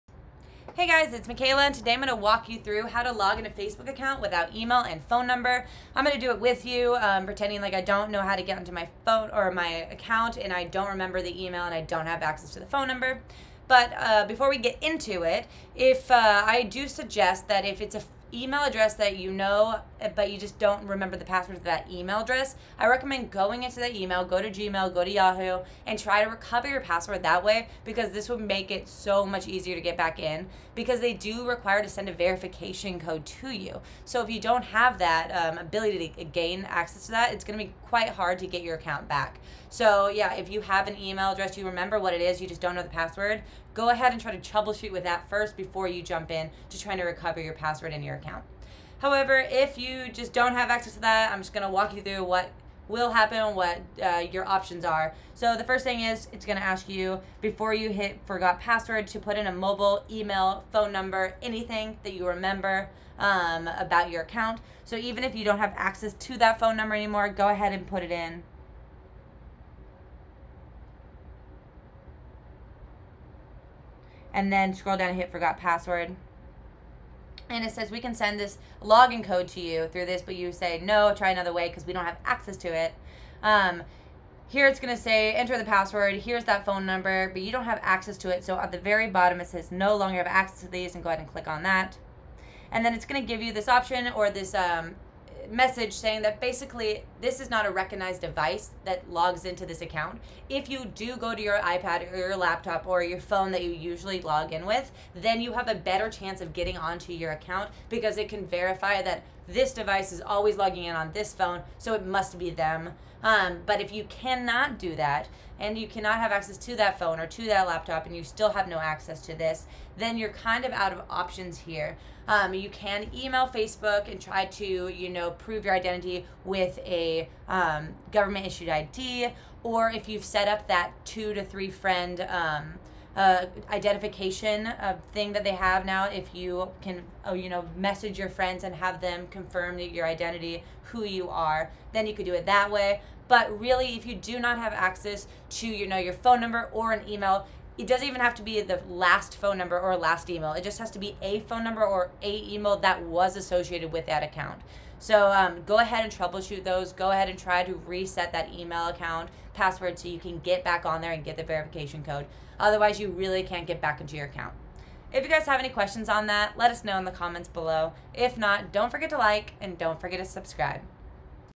How To Login Facebook Account Without Email And Phone Number
Let's login to your Facebook account without access to your email or phone number in this quick and easy guide.

Facebook gives you lots of different options to login to your account if you forgot your password. If you don't have access to either one of these the best bet is to try to go into your email and select forgot password and try to recover your password that way.

If you can't get access to your email account you can try the friend option to recover your account if you set it up so that your friends can recover your account for you. Otherwise, you'll have to submit a help request with Facebook and upload a photo ID that's government-issued and hope that Facebook will restore your account so you can login again. These steps work the same for both iPhone and Android in 2021.